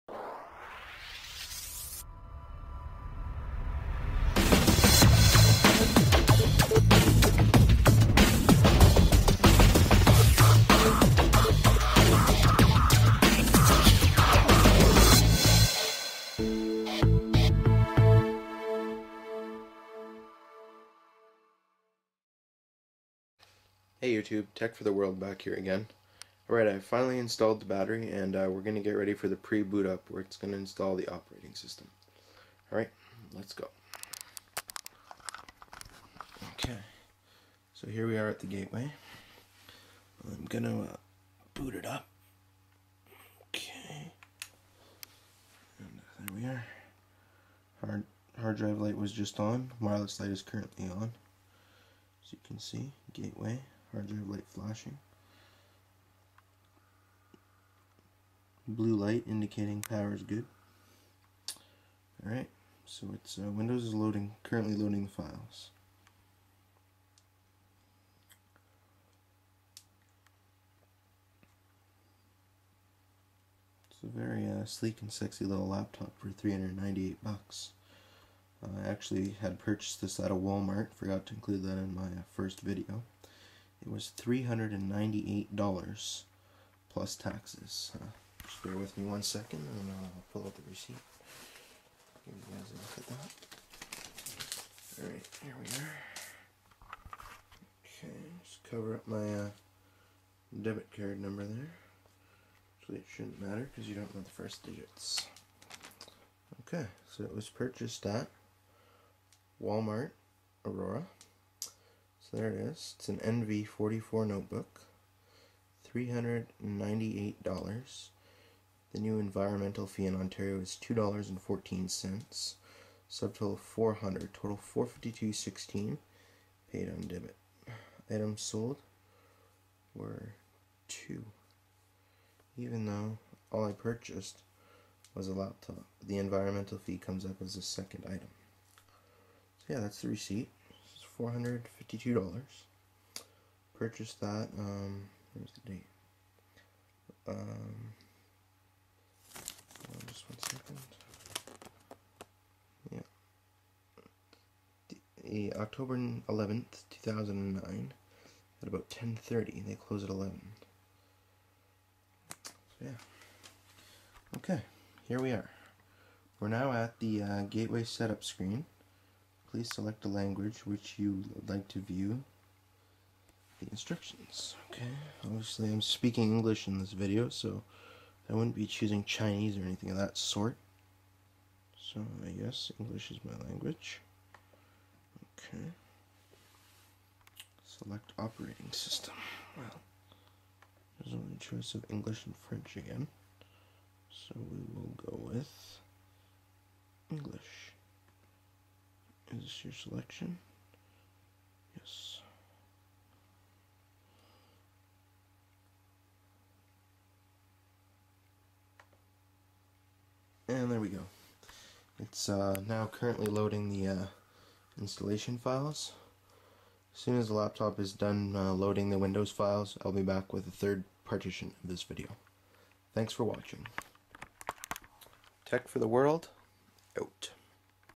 Gateway NV4403H 1st Boot
Installing The Software onto the laptop.